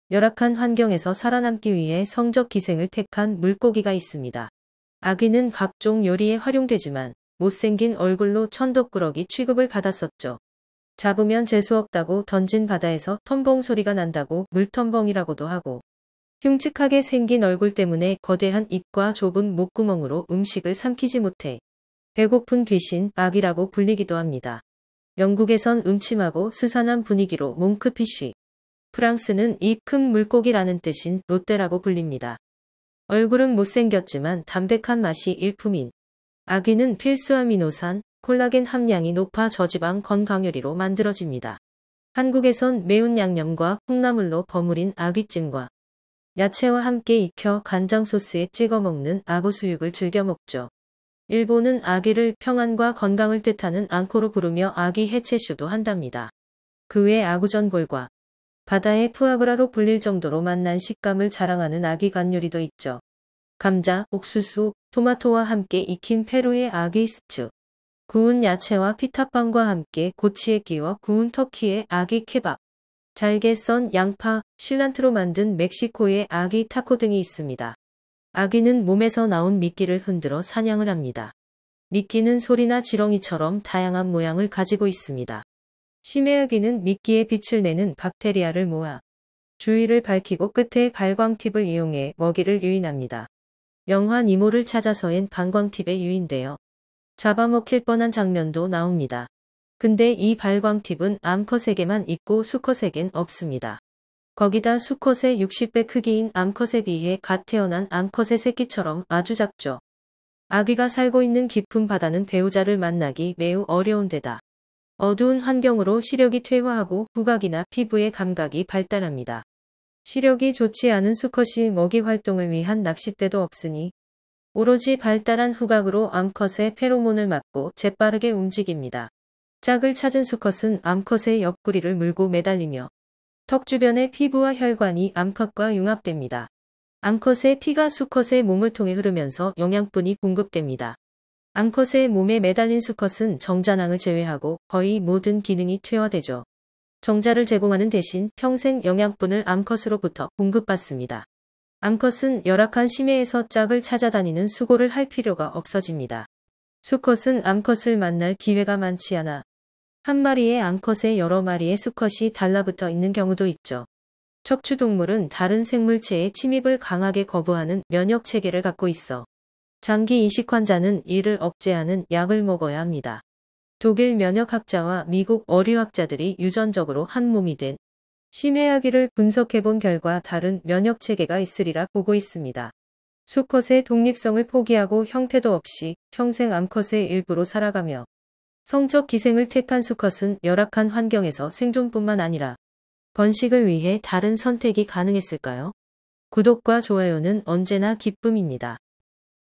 생존을 위해 암컷의 기생충이 된 심해아귀
독립을 포기하고 평생 암컷에 기생하며 살아가는 수컷아귀